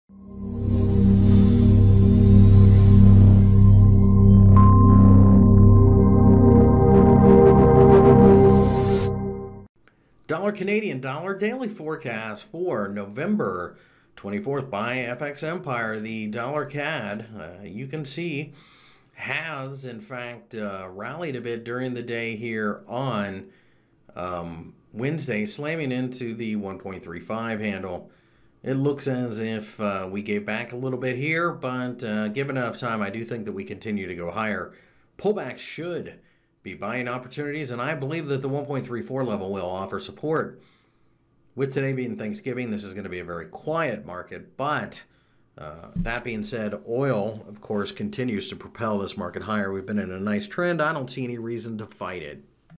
USD/CAD Technical Analysis for November 24 2016 by FXEmpire.com
- November 24 2016 currency daily technical analysis for the USD/CAD pair. Forex Technical Analysis Forex Fundamental Analysis and Forex Brokers on FX Empire